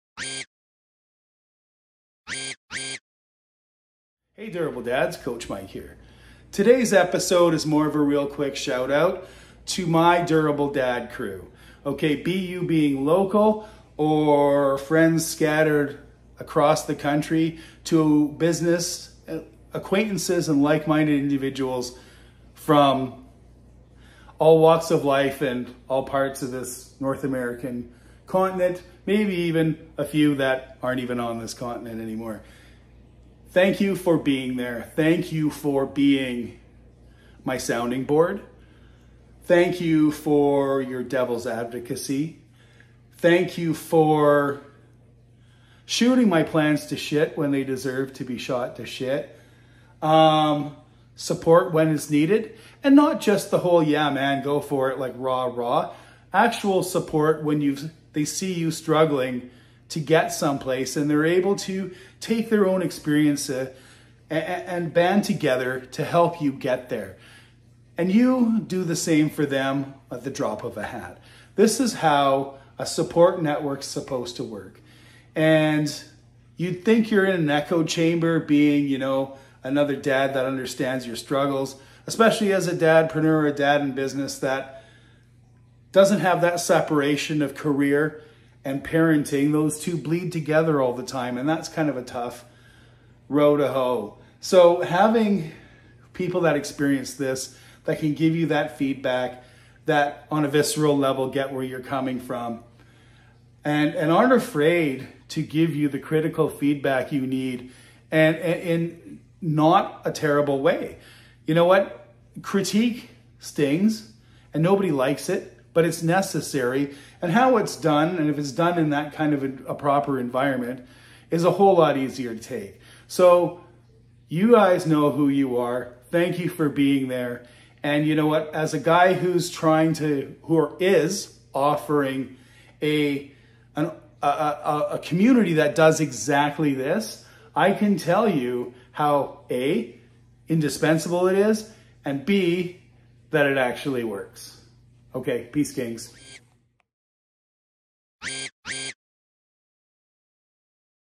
Shout out - to my Durable Dads
Here's me sending out some love to my Durable Dads crew for the help in being like-minded advisors, devil's advocate, sounding boards, and impromptu focus group. This has been indispensable to me...and will be for you too. And this is what Durable Dads is all about